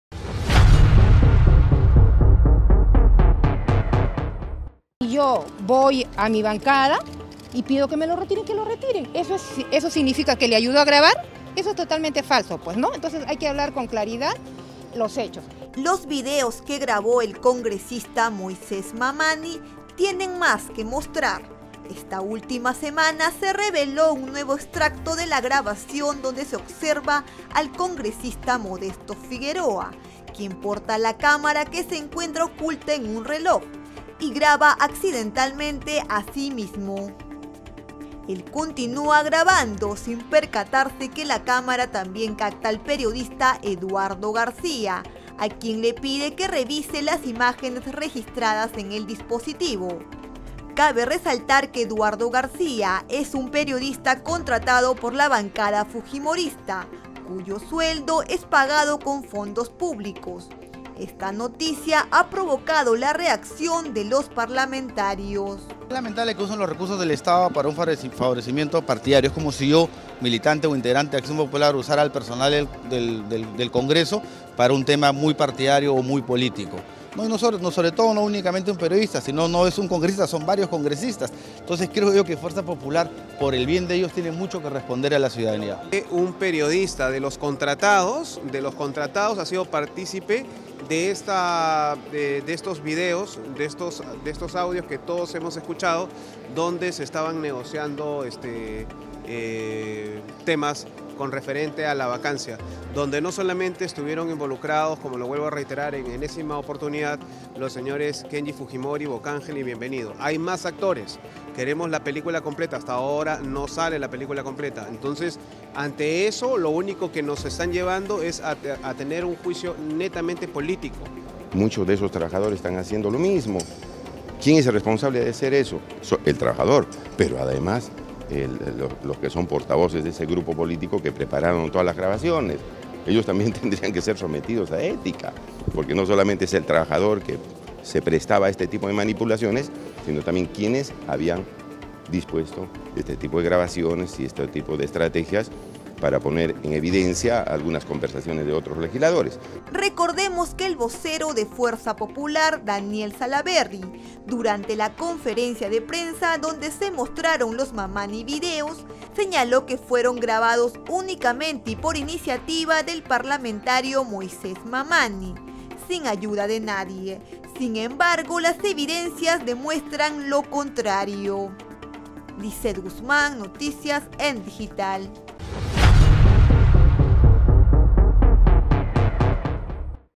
MAMANI SÍ RECIBIÓ AYUDA PARA GRABAR
[Política] EL PERIODISTA EDUARDO GARCÍA, CONTRATADO PARA TRABAJAR CON LA BANCADA FUJIMORISTA, HABRÍA APOYADO AL PARLAMENTARIO MOISÉS MAMANI PARA GRABAR LOS VÍDEOS QUE PROVOCARON LA VACANCIA DE PEDRO PABLO KUCZYNSKI.